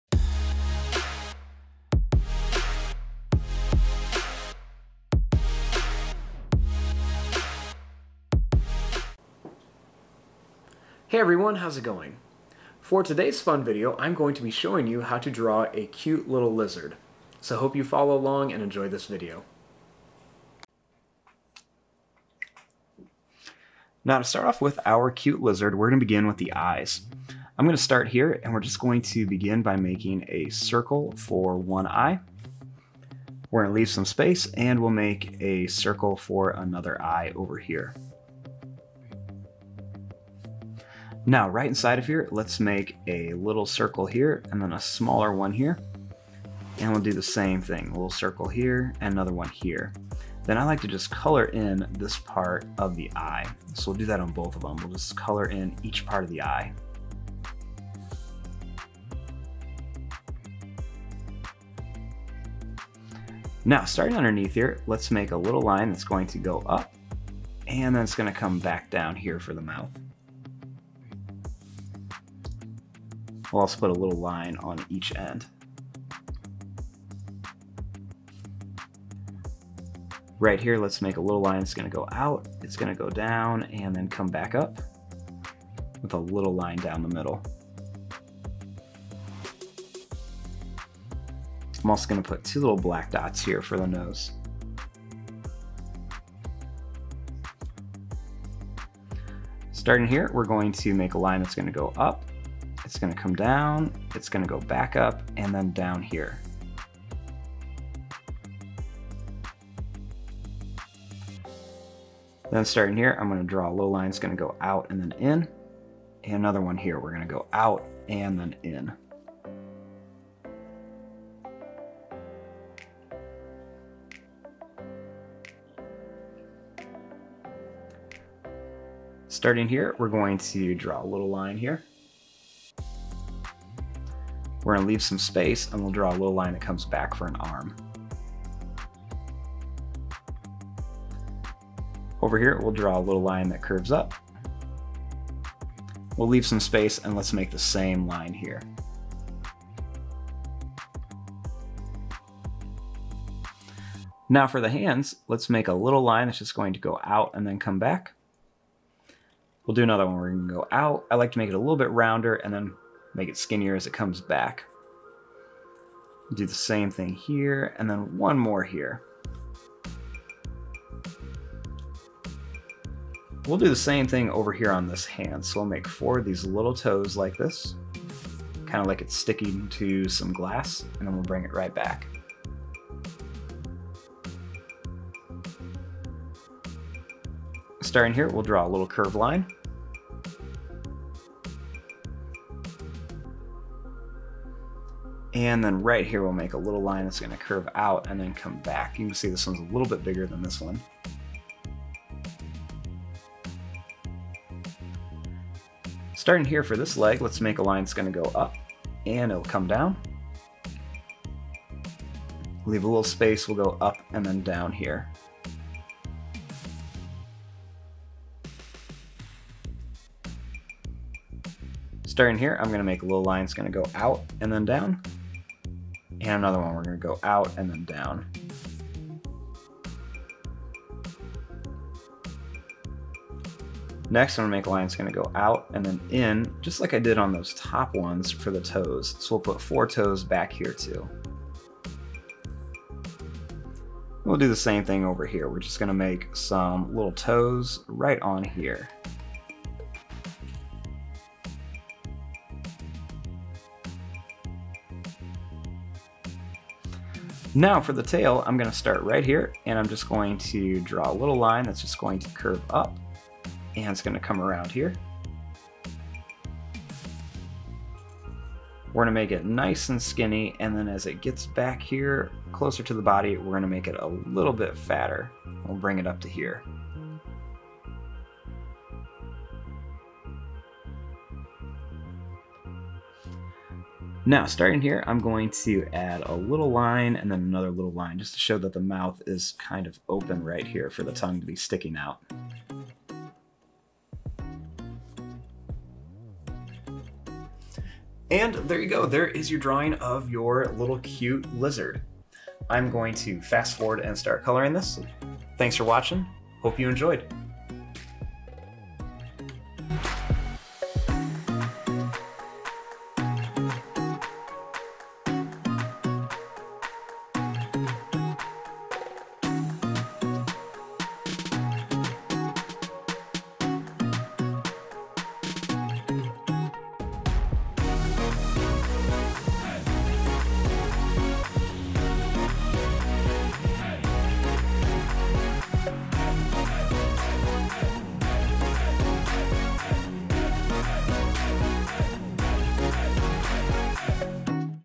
How to Draw a CUTE LIZARD
Step by step video on how to draw a CUTE LIZARD!!!